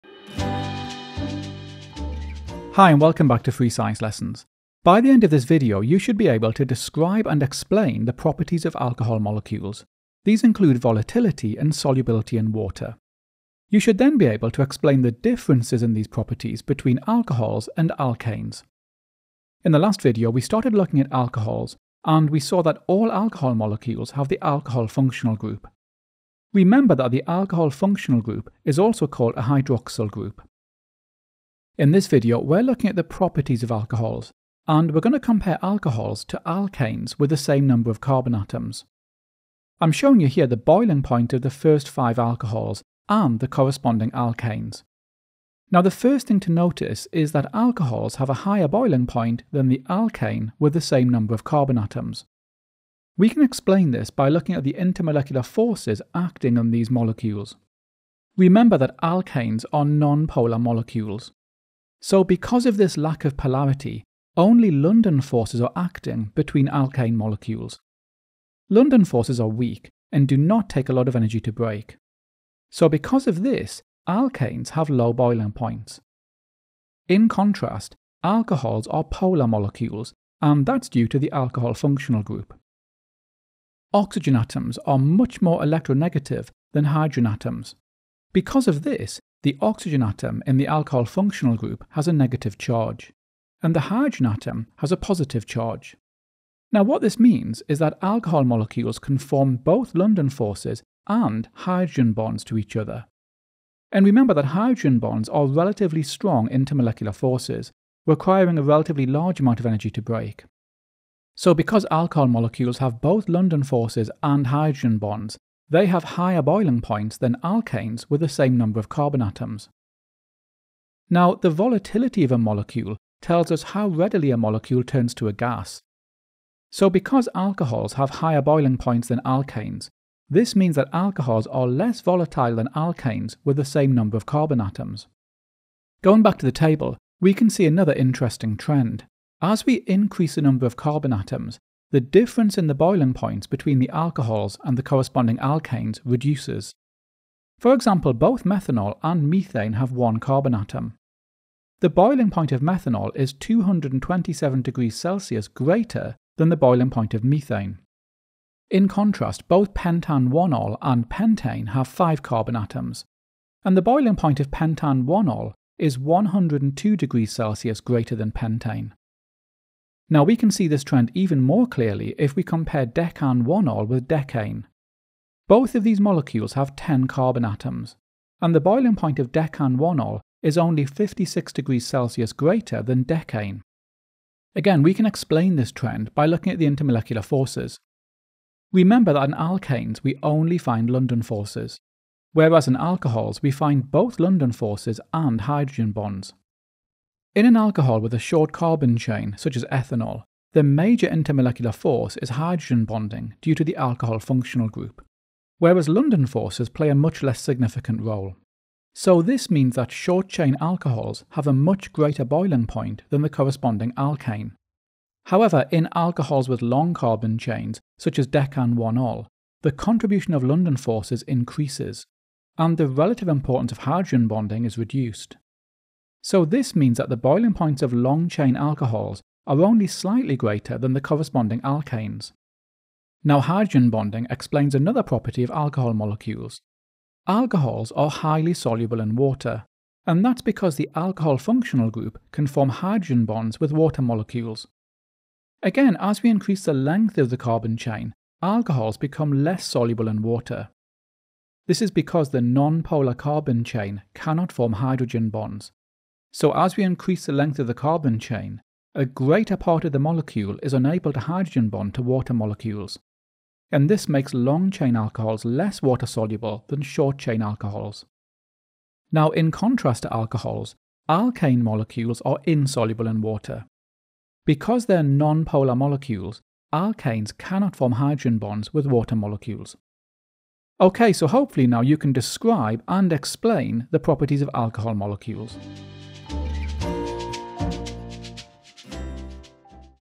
A Level Chemistry Revision "Volatility and Solubility of Alcohols"
In this video, we look at the volatility and solubility of alcohols. First we look at the boiling points of alcohols and how these compare with equivalent alkanes, linking this to volatility. We explain this by looking at the intermolecular forces involved. We then look at the water-solubility of alcohols compared to alkanes, again in terms of intermolecular forces.